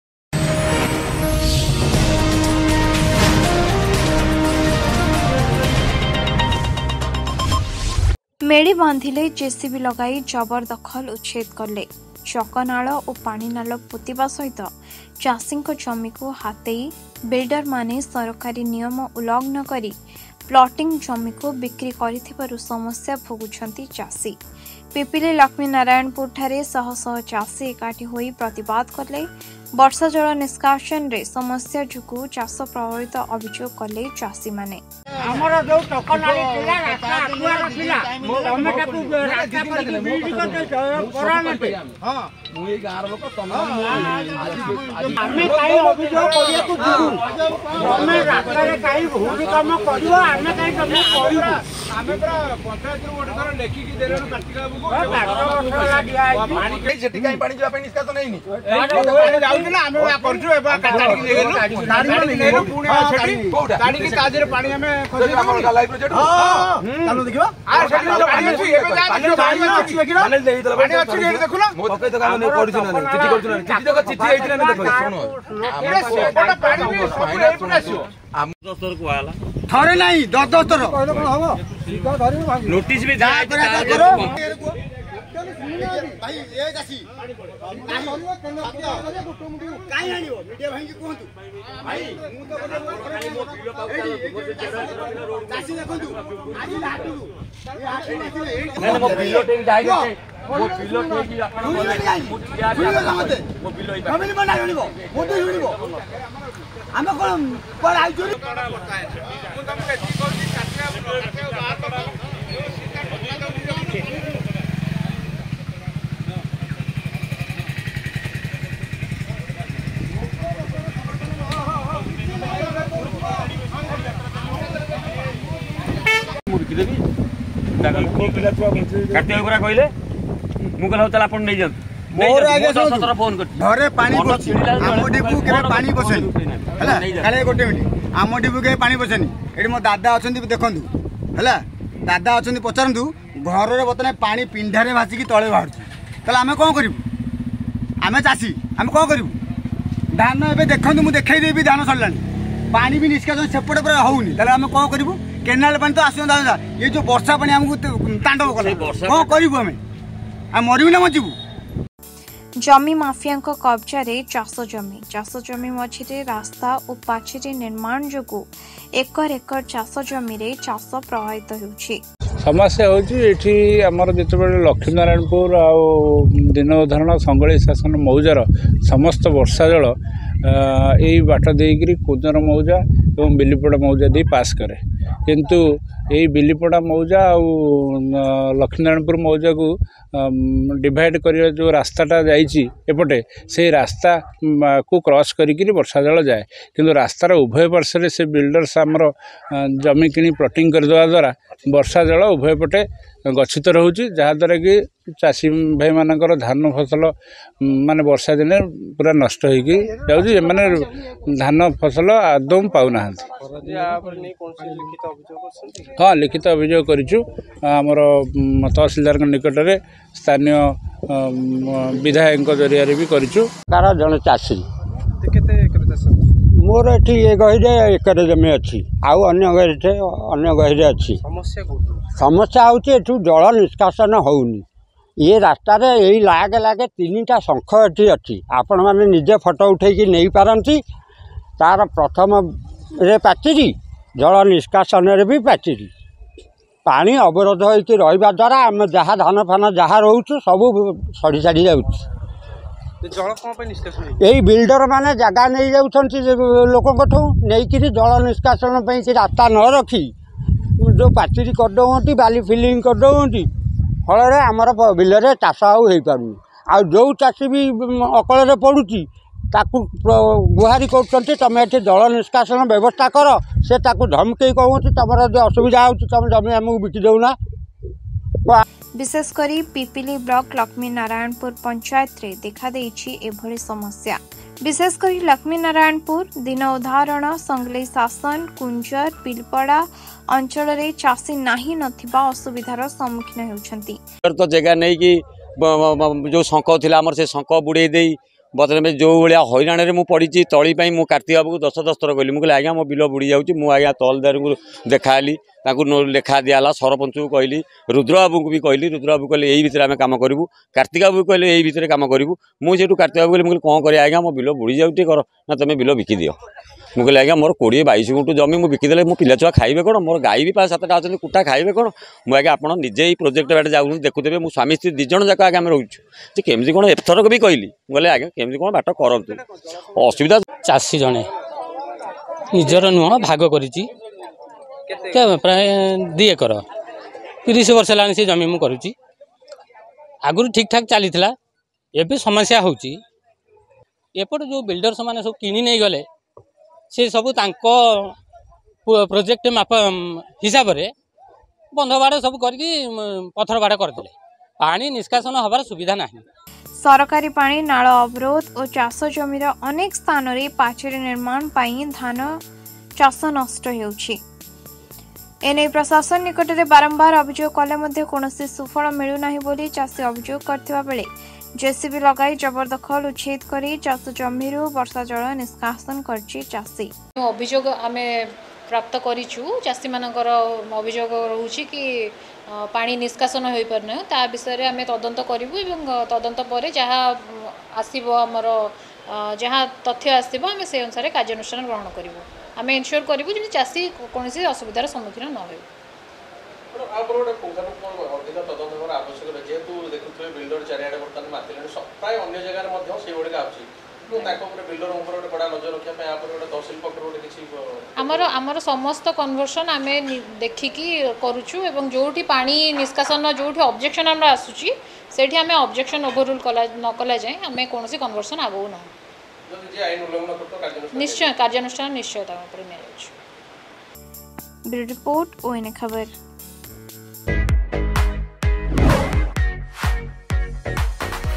Pipili: Agricultural land under the control of land Mafia || ONA KHABAR
Pipili: Agricultural land under the control of land Mafia
About Channel:
ଚ୍ୟାନେଲ୍ ବିଷୟରେ.